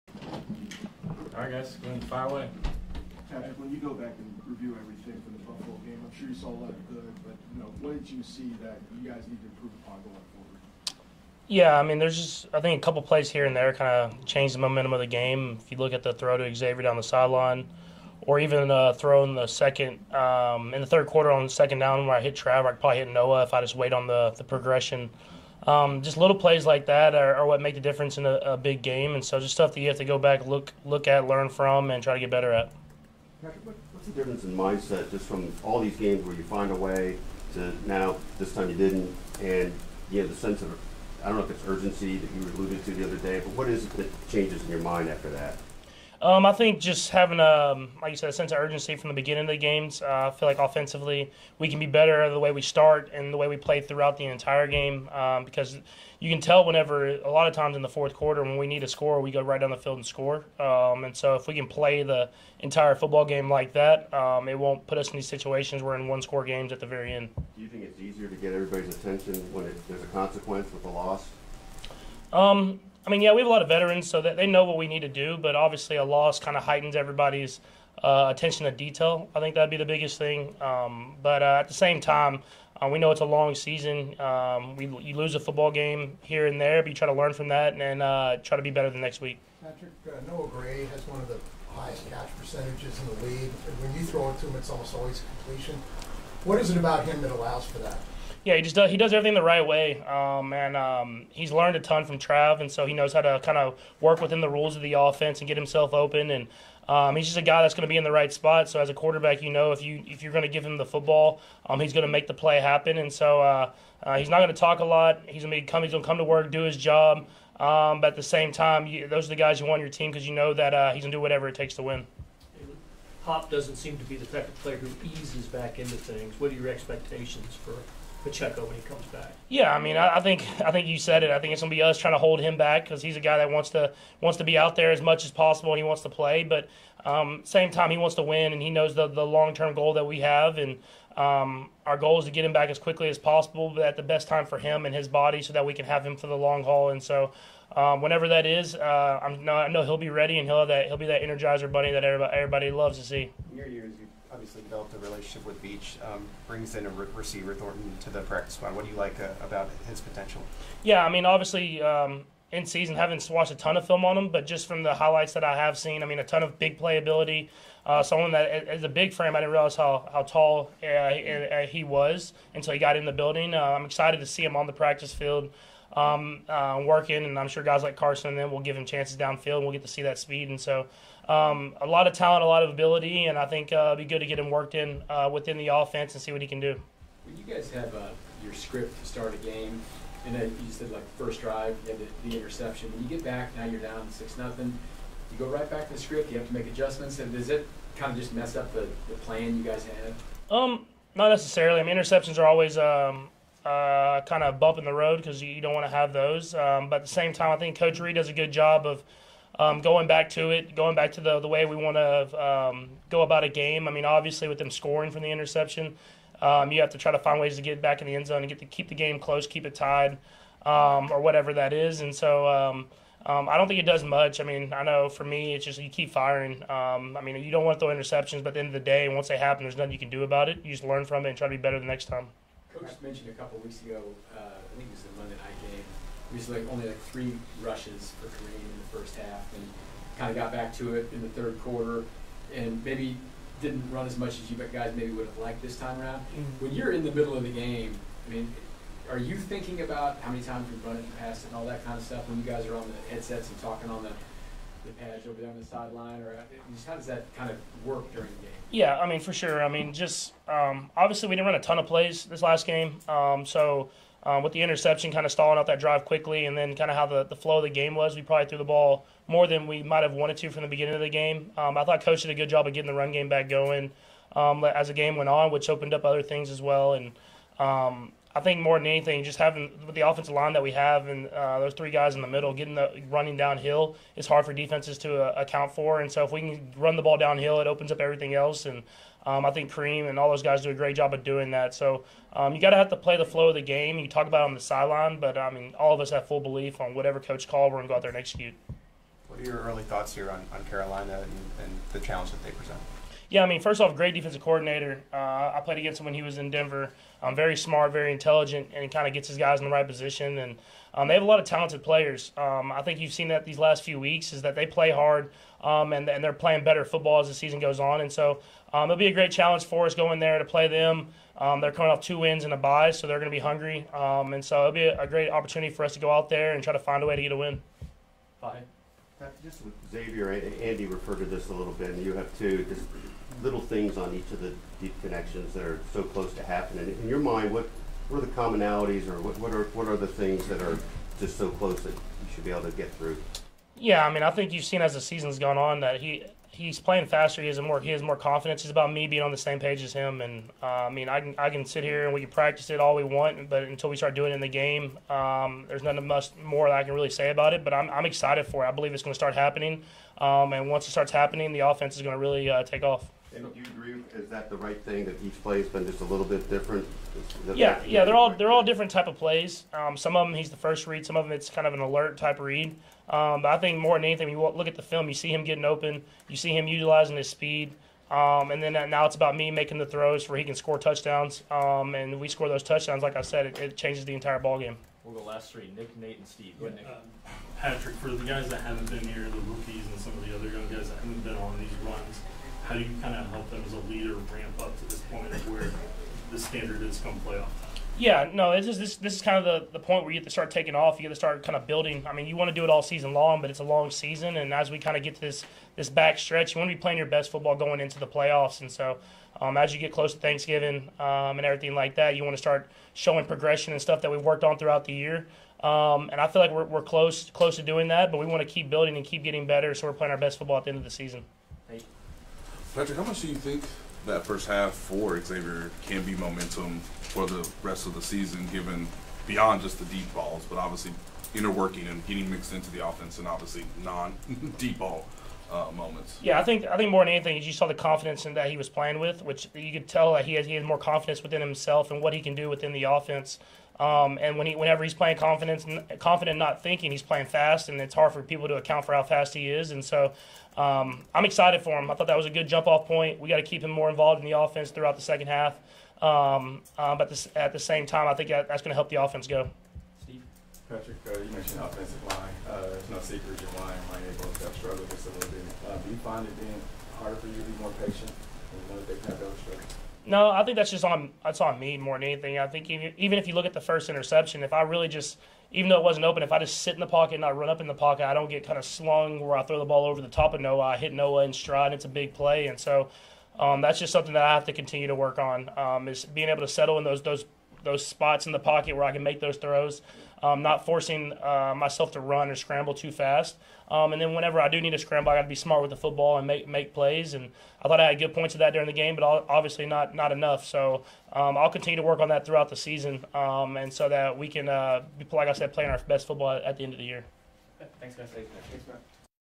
Chiefs QB Patrick Mahomes says a loss 'heightens everybody's attention to detail'
Chiefs QB Patrick Mahomes spoke with the media before practice on Wednesday, Nov. 20.